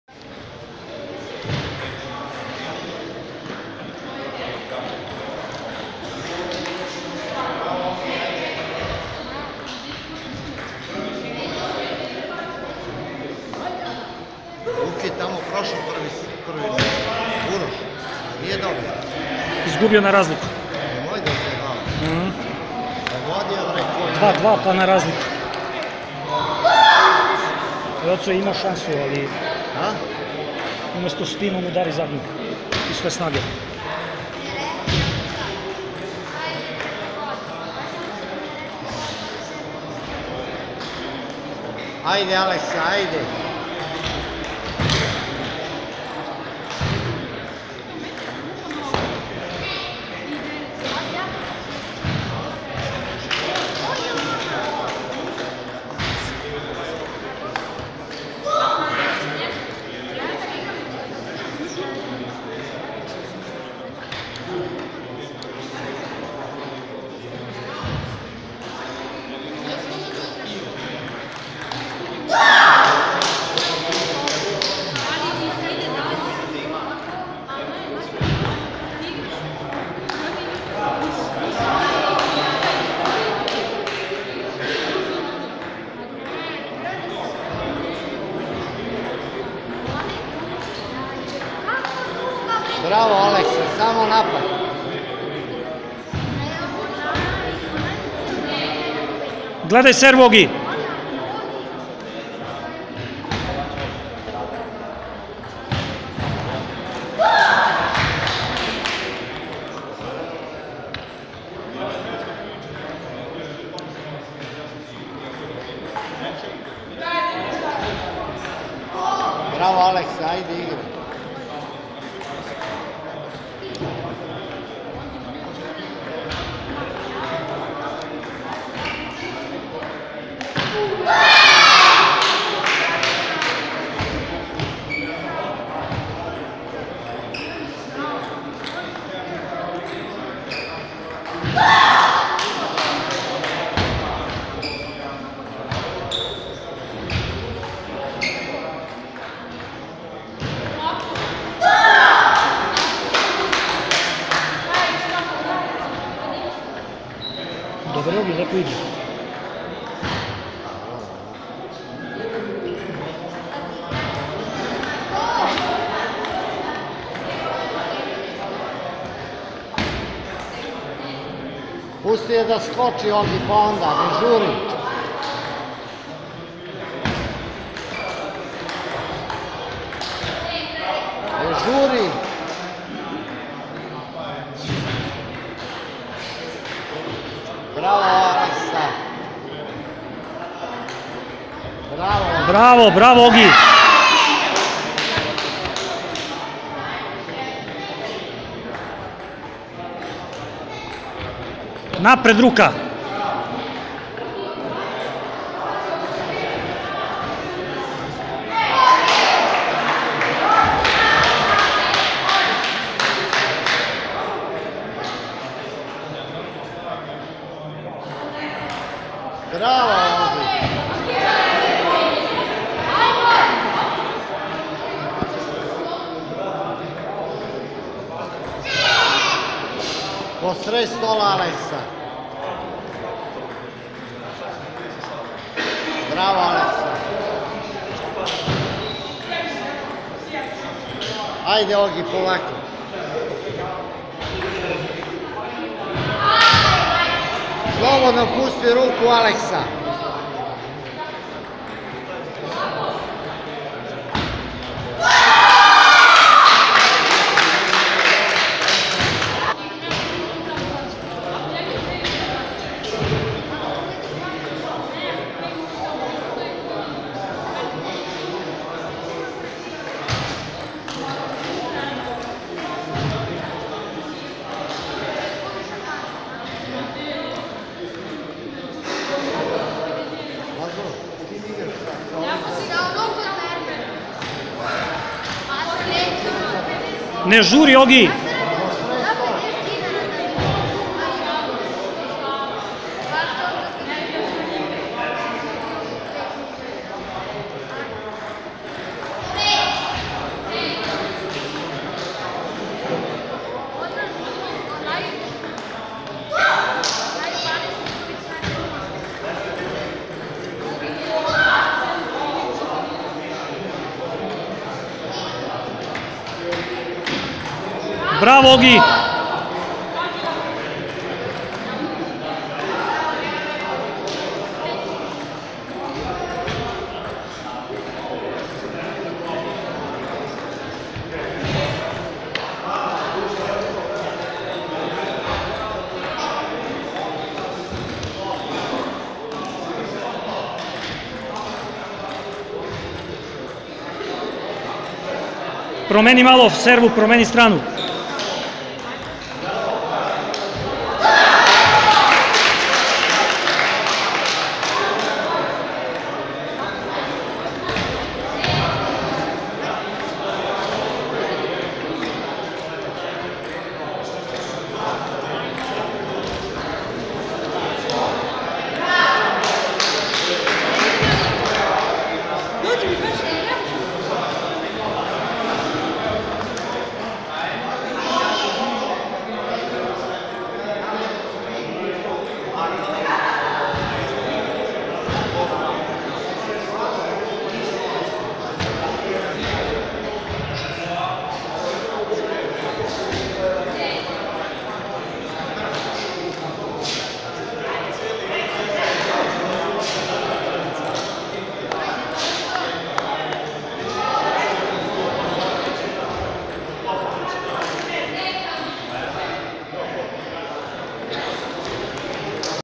Ognjen Stefanovic
Mec u kategoriji dece uzrasta 5-8 razred na prvomajskom turniru odrzanom dana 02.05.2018 godine u organizaciji STK Napad iz Krusevca.